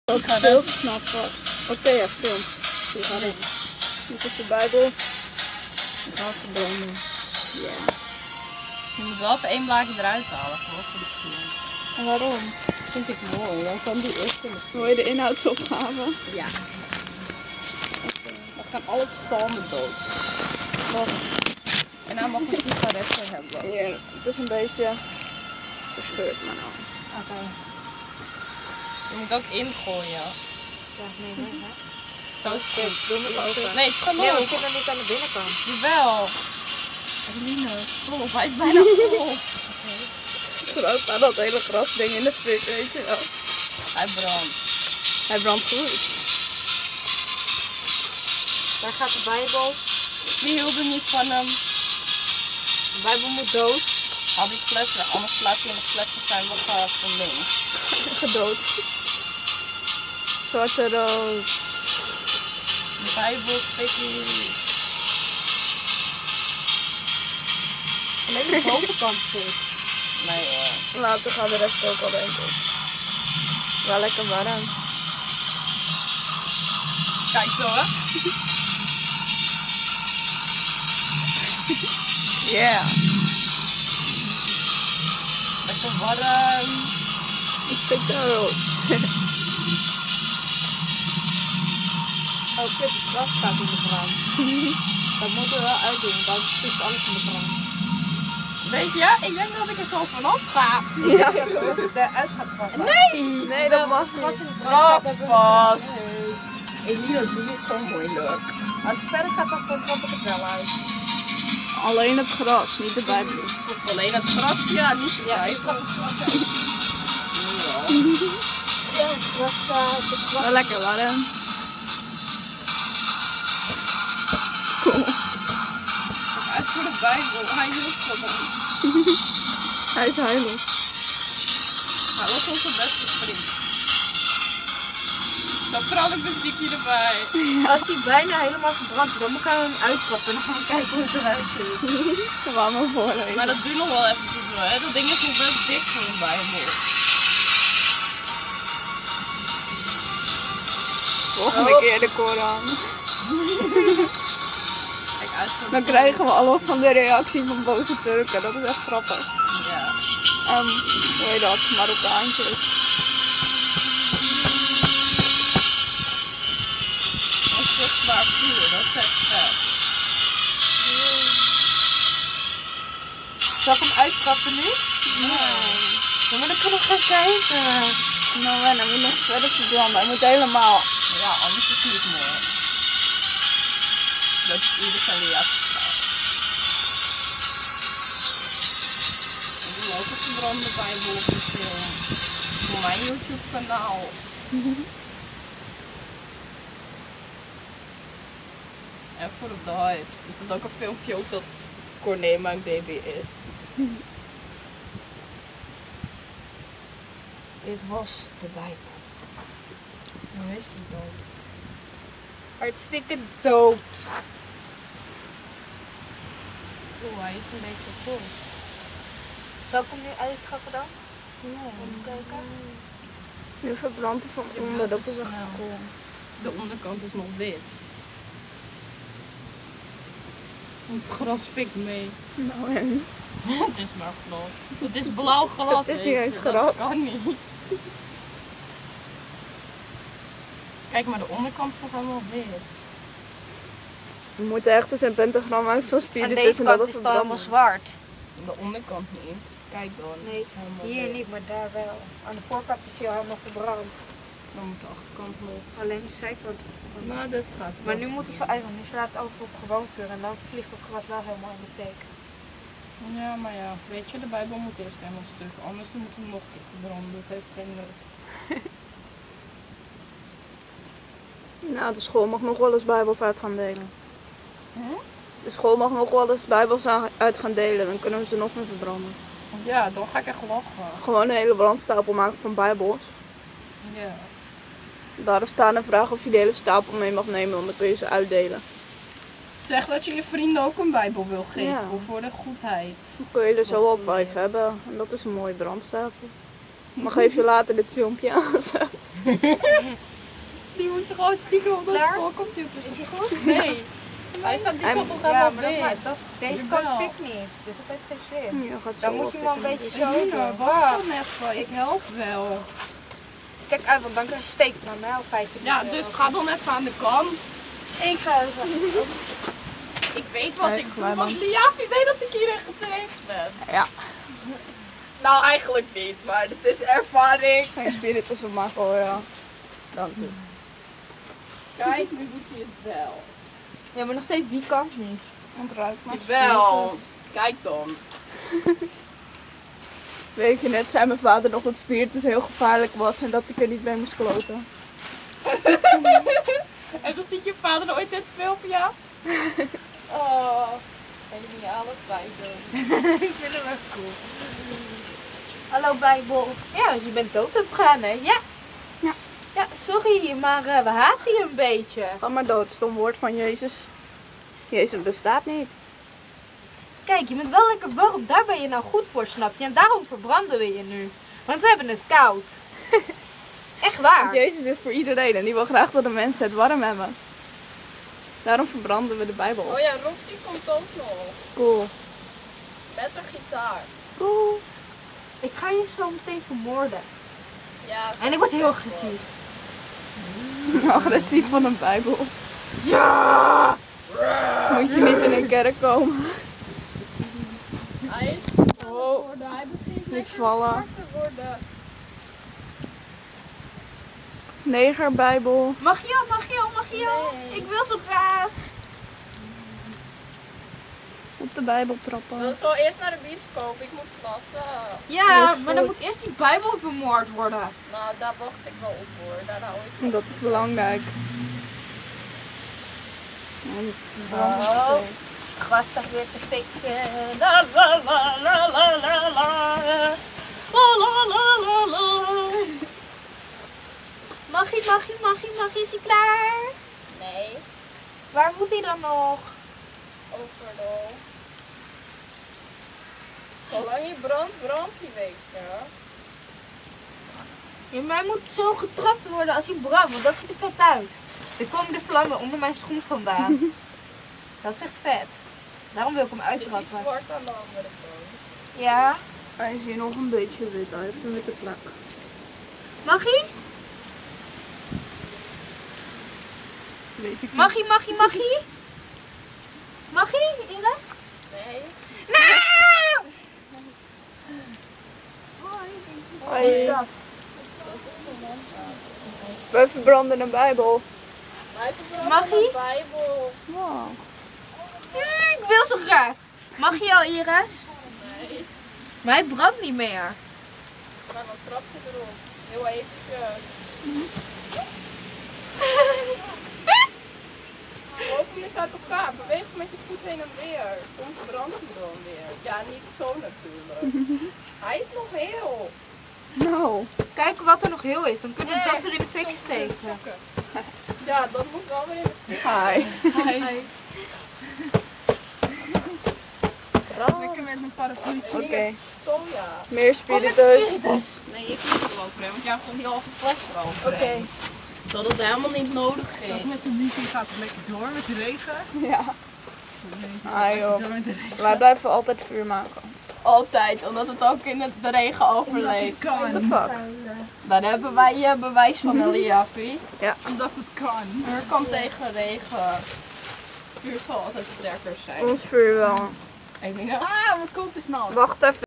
Meeting HHW - Bijbel verbranden! [1/2]
Yaaaaay xD deel 1 van 2; bijbel verbranden op meeting HHW. Ander deel komt nog xD